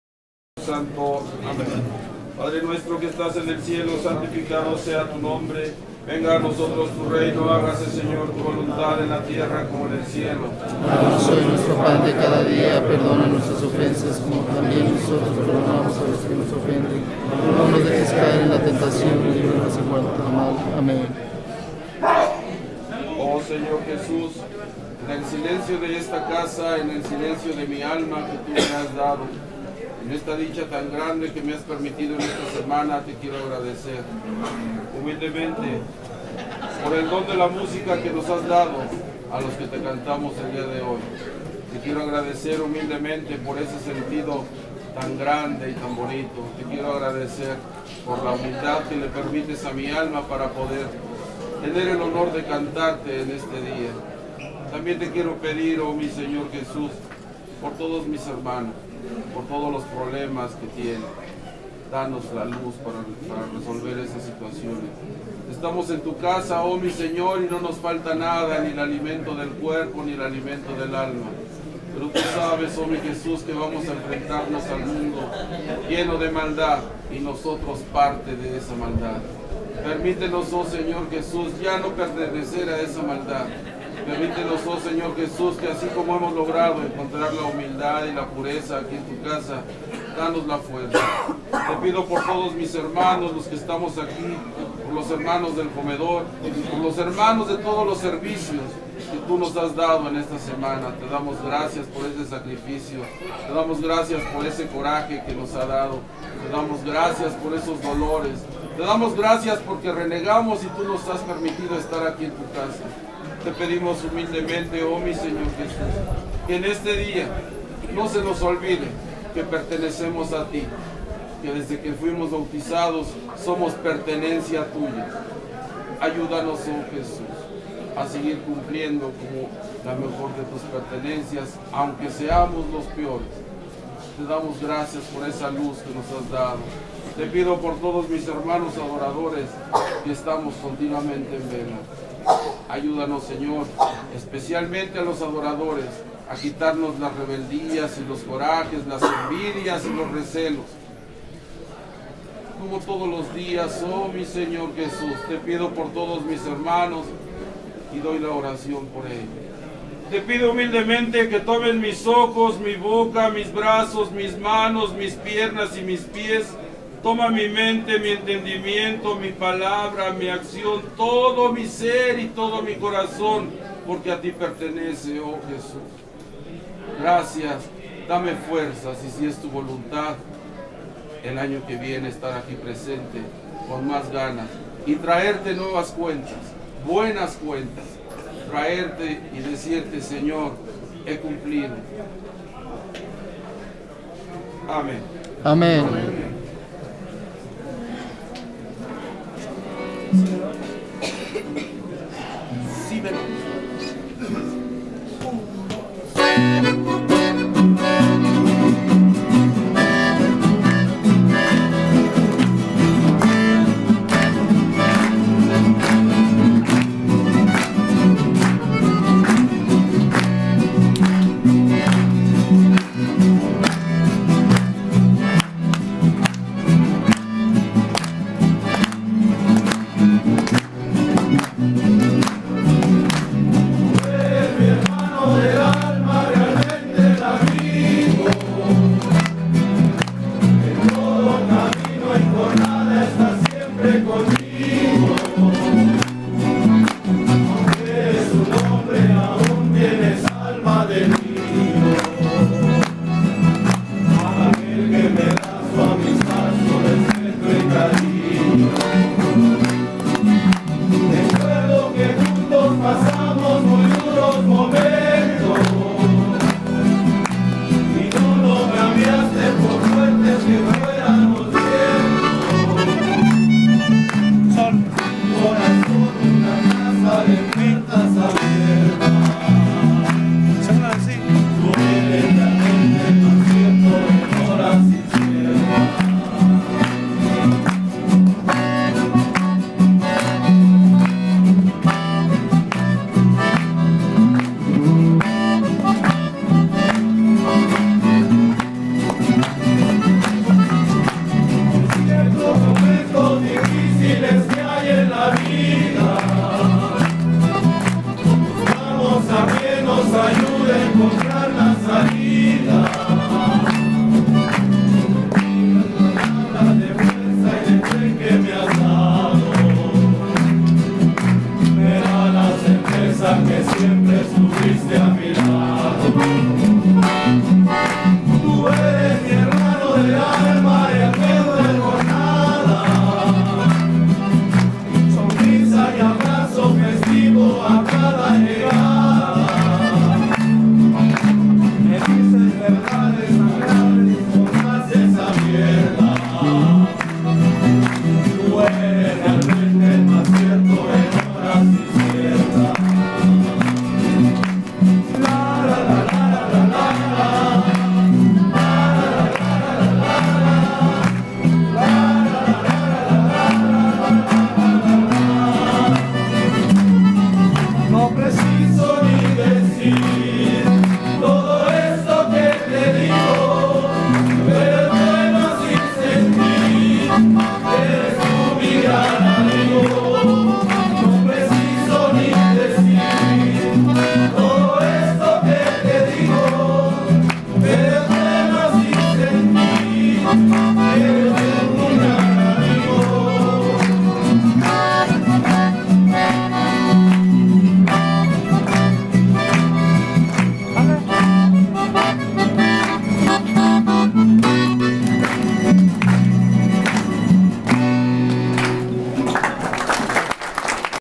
ORACION HNO. MIGUEL
EMOTIVA ORACION POR PARTE DEL HERMANO MIGUEL, LO TOMA EL ESPIRITU SANTO Y SE SIENTE LA PRESENCIA DEL SEÑOR, SENSACION QUE TRANSMITE A TODOS LOS QUE ESTAMOS REUNIDOS EN EL NOMBRE DEL SEÑOR.